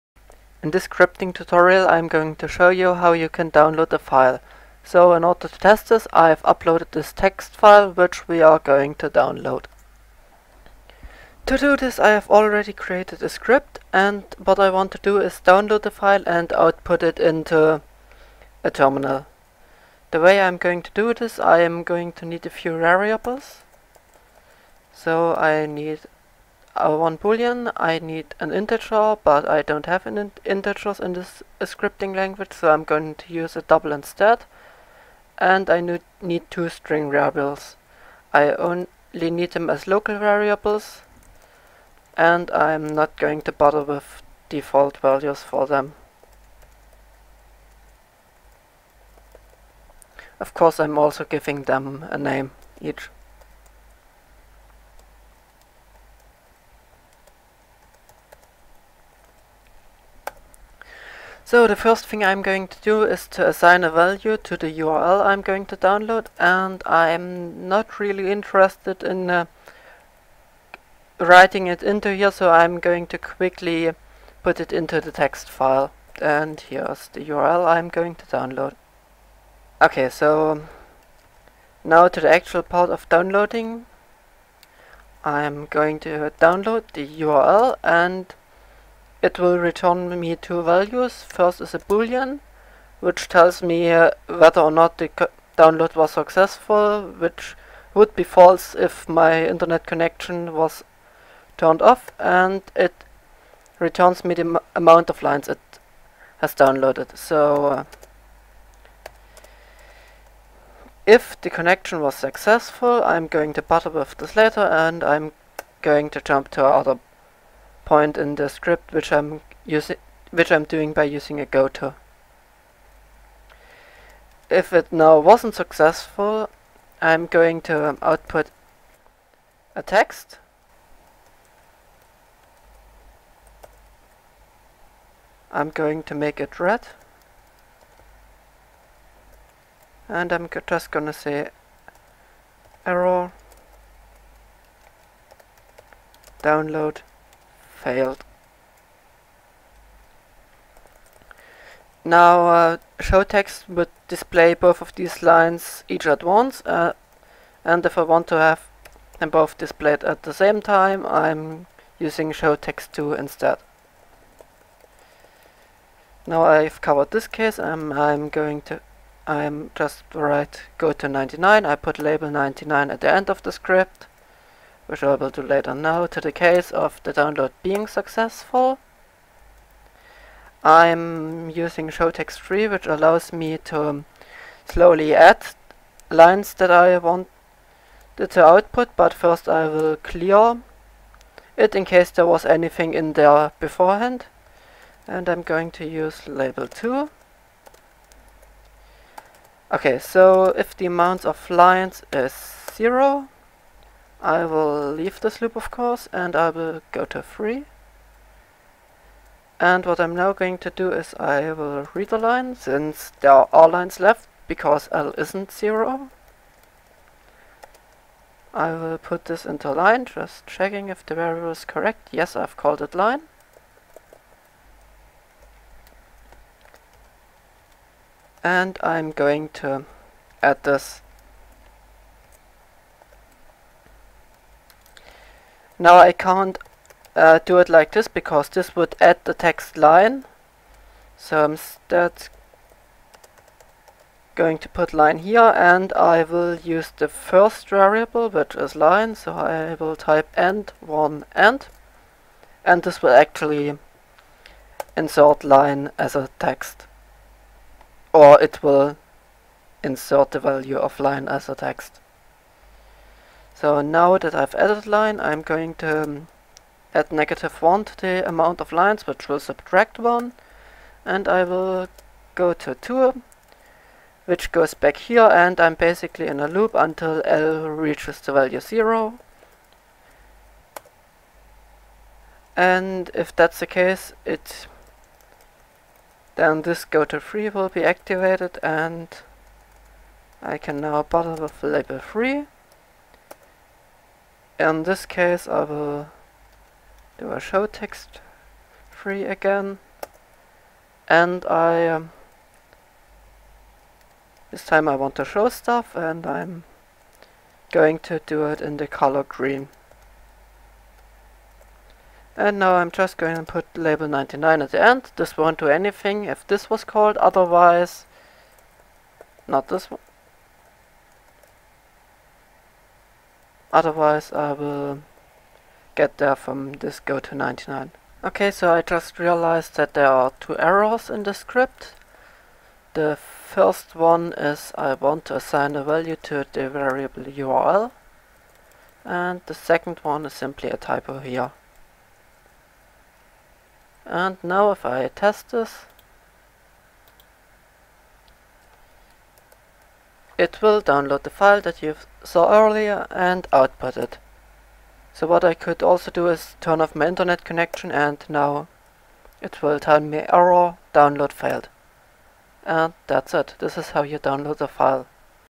File Download | VVOC Scripting Tutorial
VVOC is the turing complete scripting language in VVOVOV.
In this Video I am going to show you how you can download a file in this scripting language.